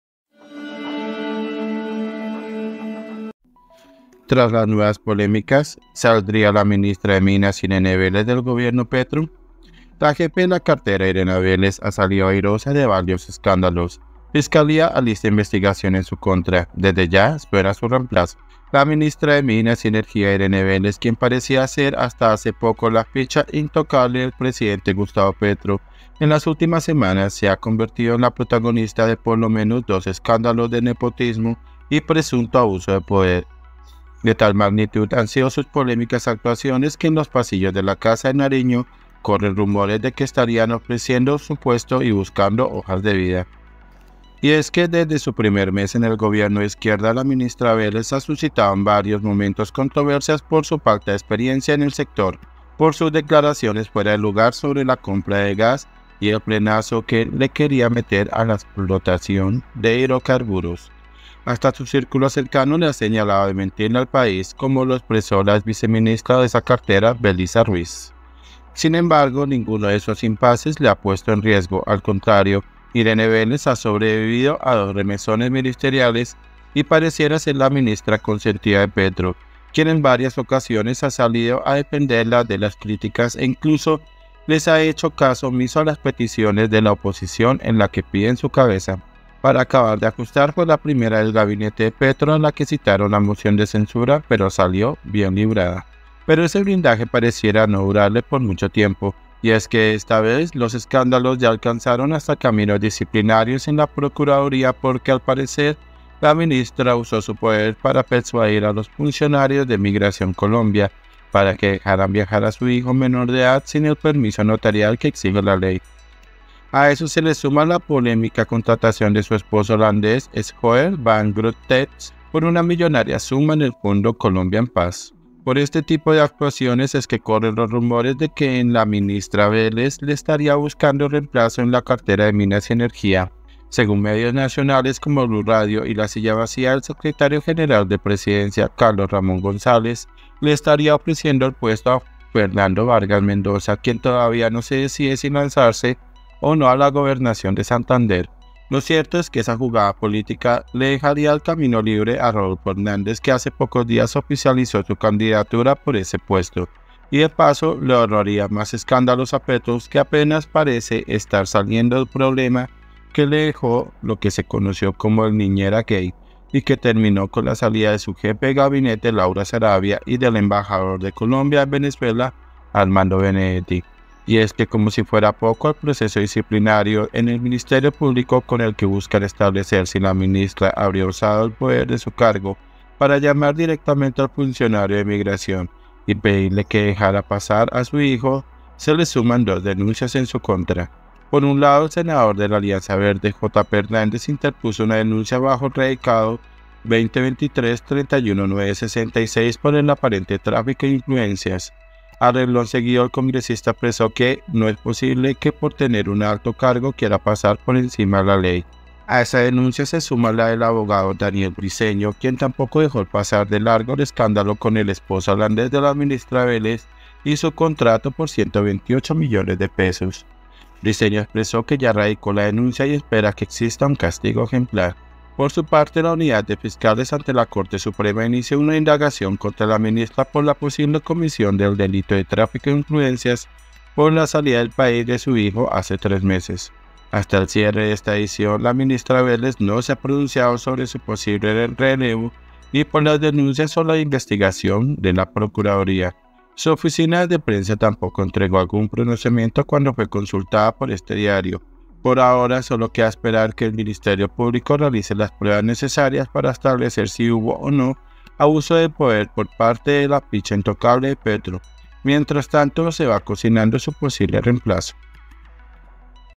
🔴 TRAS LAS NUEVAS POLÉMICAS ¿SALDRÍA LA MINISTRA IRENE VÉLEZ DEL GOBIERNO PETRO?
La jefe de la cartera Irene Vélez, ha salido airosa de varios escándalos. Fiscalía alista investigación en su contra. Desde ya suena su reemplazo.